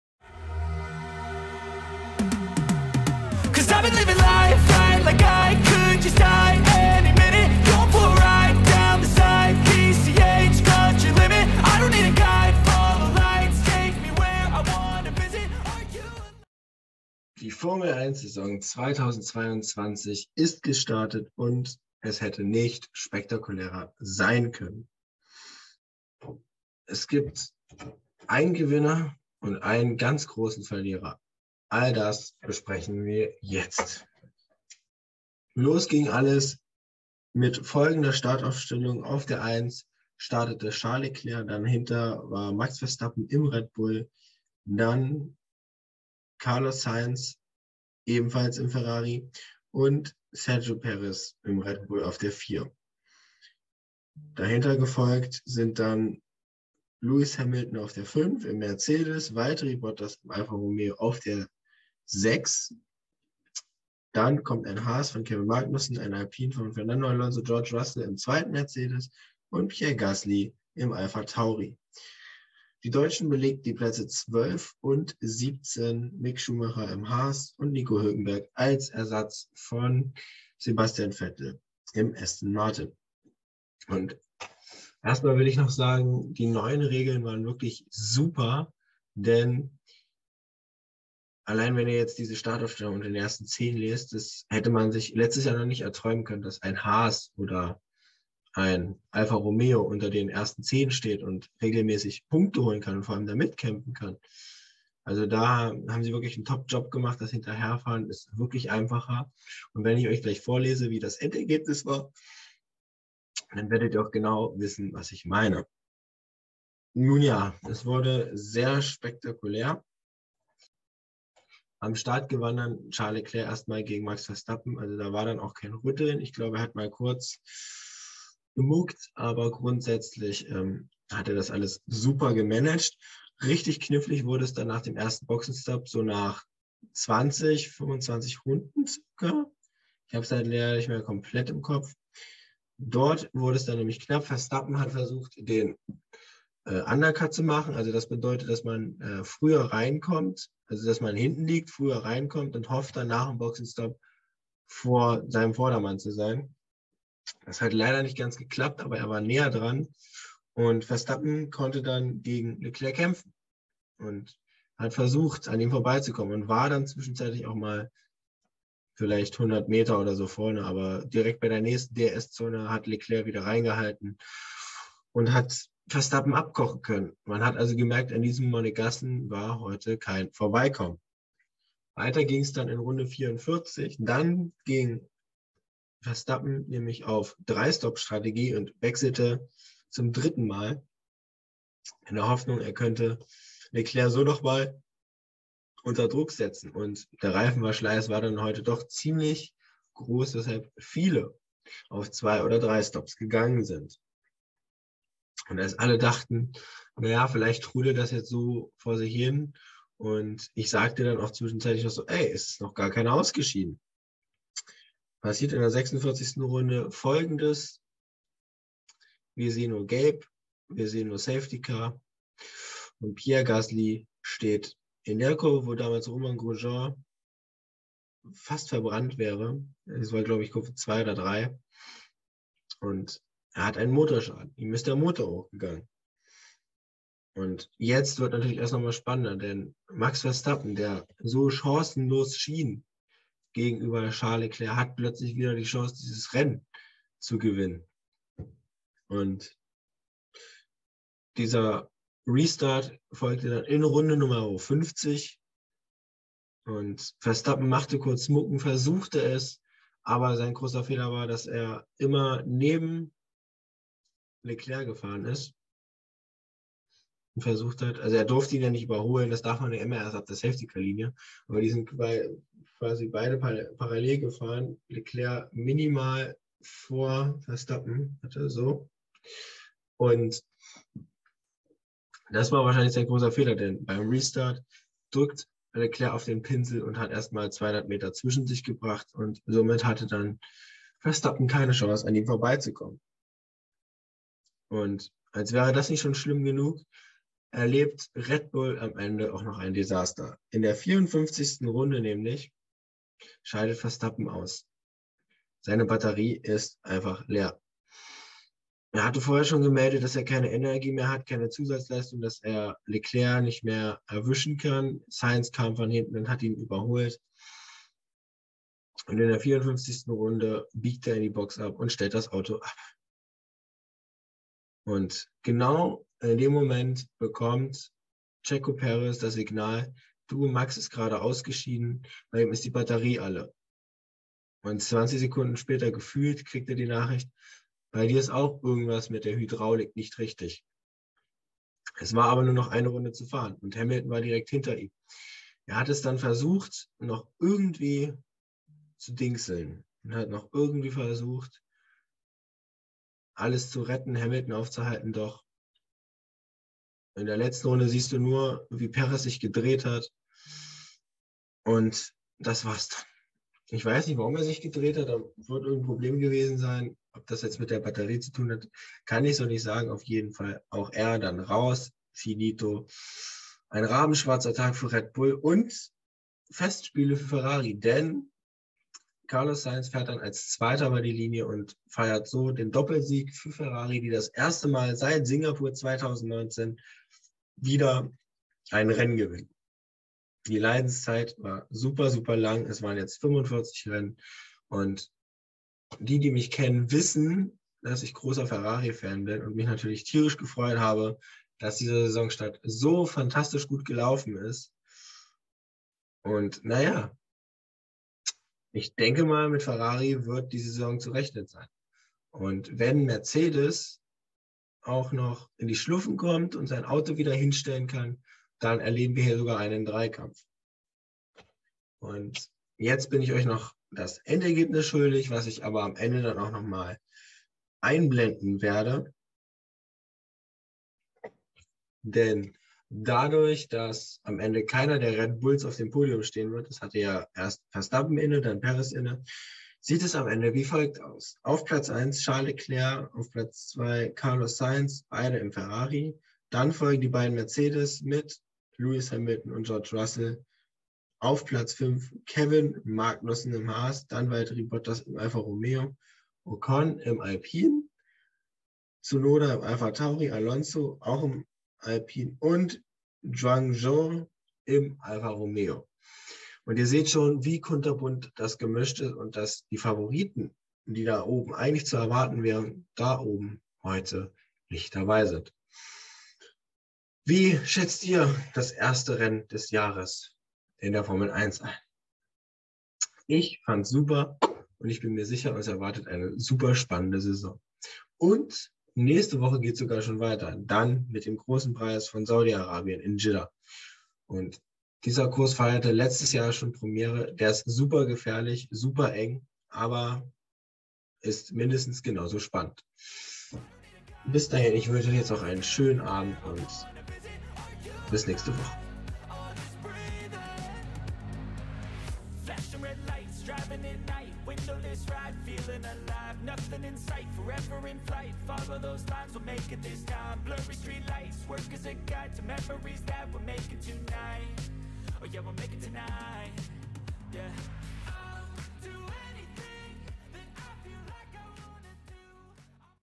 Ferrarifestspiele zum Auftakt 🥰|BahrainGP|keep.racing#002
Die Formel 1 Saison 2022 ist gestartet und es hätte nicht spektakulärer losgehen können.
Der große Gewinner am Ende heißt Ferrari die nach Singapur 2019 endlich mal wieder (doppelt) ganz oben stehen!

Für red Bull hätte das Wochenende nicht schlimmer sein können. Statt einem Podestplatz gab es für beide Fahrer am Ende 0 Punkte und die großen Fragezeichen warum die Hydraulik gestreikt hat.
Nächstes Wochenende geht es dann direkt in Jiddah schon weiter. In Saudi-Arabien wird sich dann zeigen ob Ferrari wirklich dauerhaft um die Spitze mitkämpfen kann und ob Mercedes, die mit Glück heute das Podium erreichten, zu alter Form zurückfinden können!